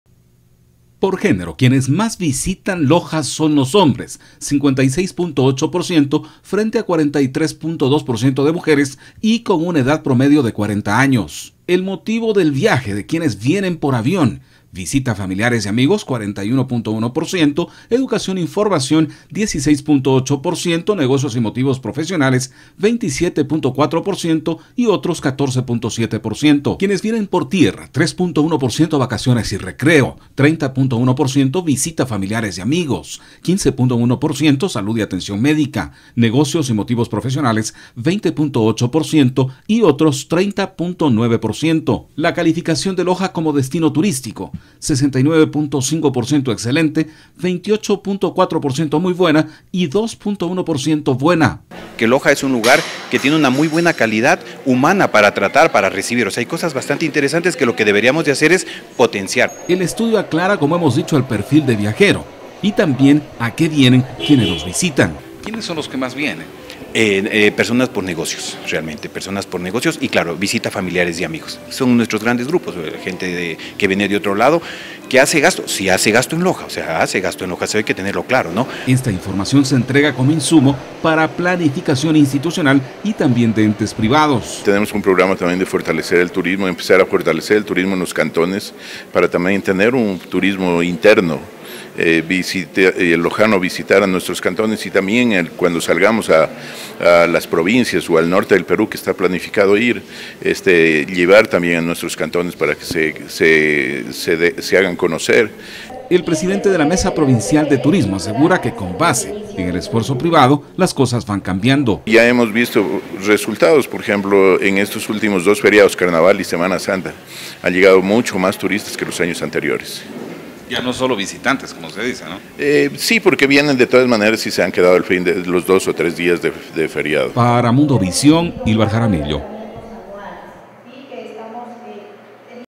Mayor número de quienes visitan Loja lo hacen por negocios y compromisos profesionales
Con datos presentados este jueves 4 de abril por el Observatorio Turístico de la UTPL, se determina que el mayor número de quienes llegan a Loja, lo hacen por negocios y compromisos profesionales. Esta información se recoge en un documento denominado “El Perfil del Visitante en Loja